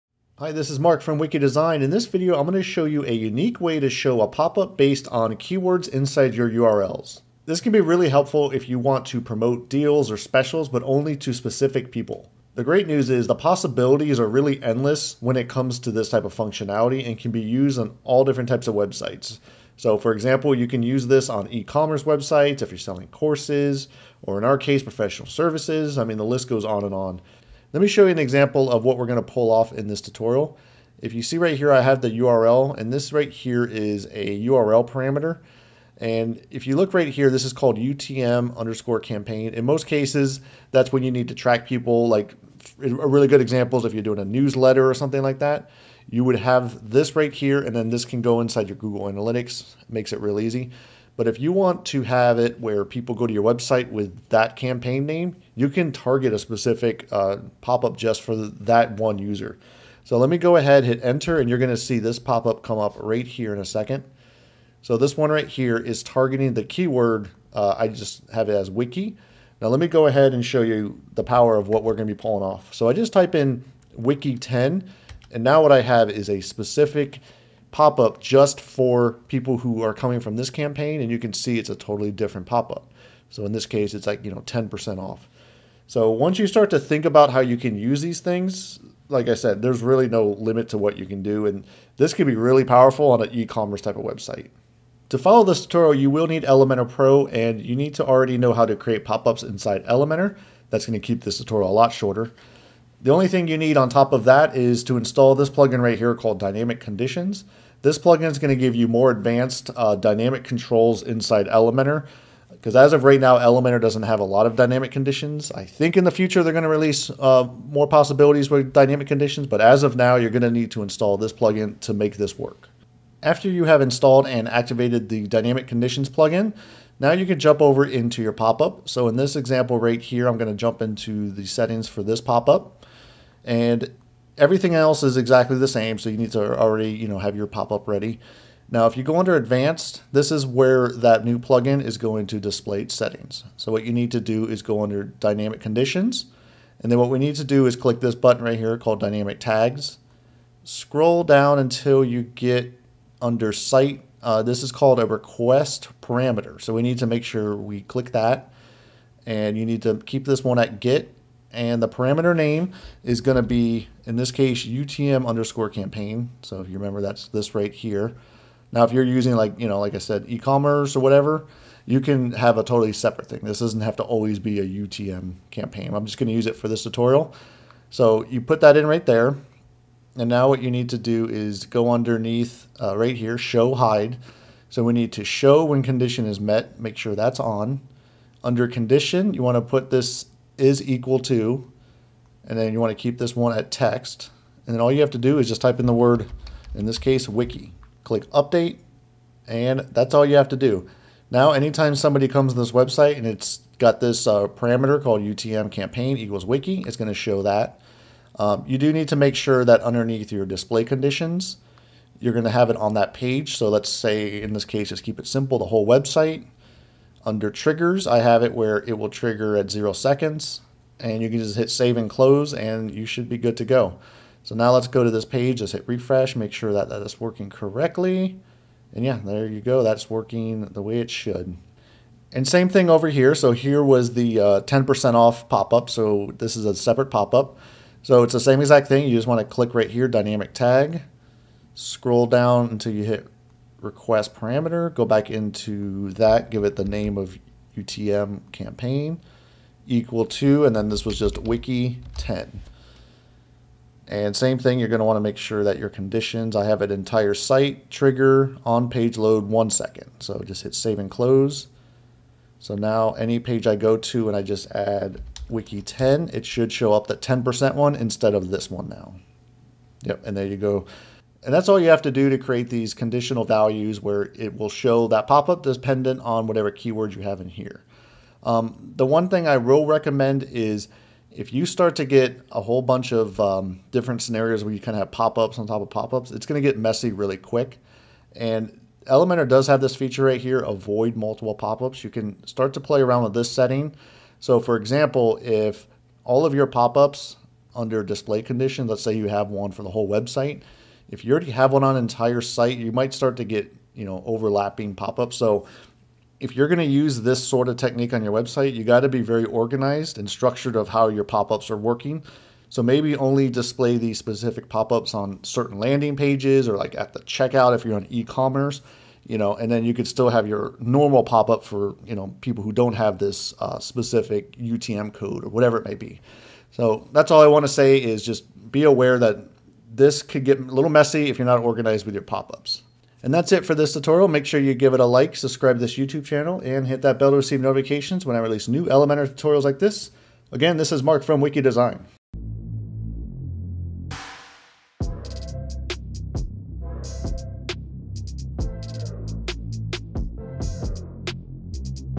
Trigger Elementor Pop Up Based on URL Parameter
In this video I will show you a unique way to display a popup based on keywords in a URL. This can be helpful if you want to promote deals or specials only to certain people.

To follow this tutorial you will need Elementor Pro and know how to create popups.

Timestamps:
0:00 Introduction
2:03 Pop Up Dynamic Conditions Settings